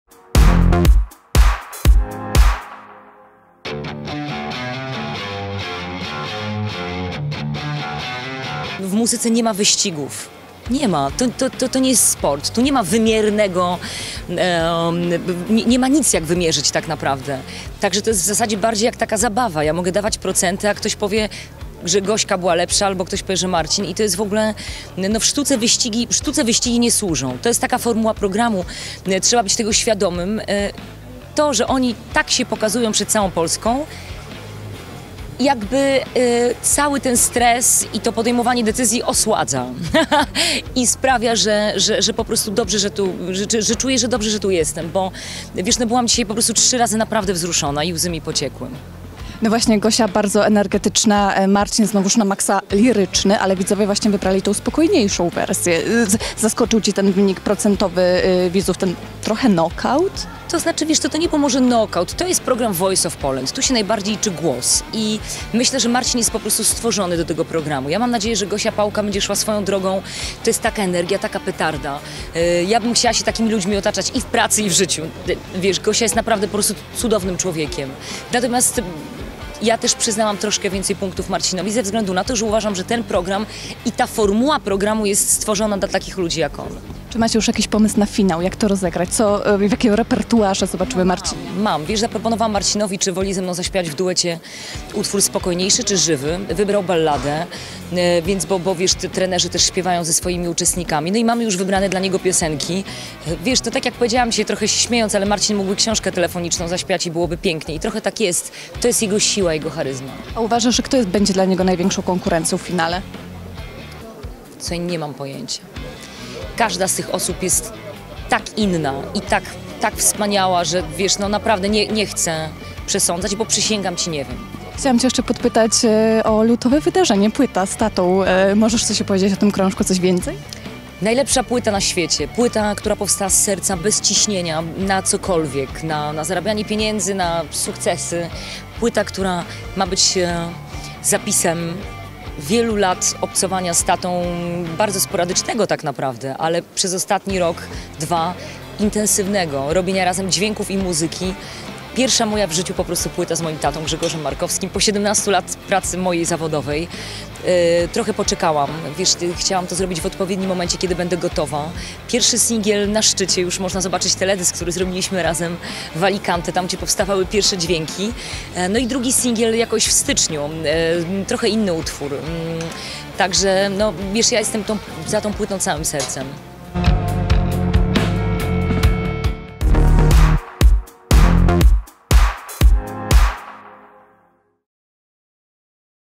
Patrycja Markowska i Marcin Sójka wygrają „The Voice of Poland”?
Patrycja Markowska doprowadziła do finału 9. edycji "The Voice of Poland" charyzmatycznego Marcina Sójkę. Czy to właśnie on wygra program i zostanie Najlepszym Głosem w Polsce? Trenerka opowiedziała nam o trudnych decyzjach w programie, przygotowaniach do finału "The Voice of Poland" oraz nowej płycie!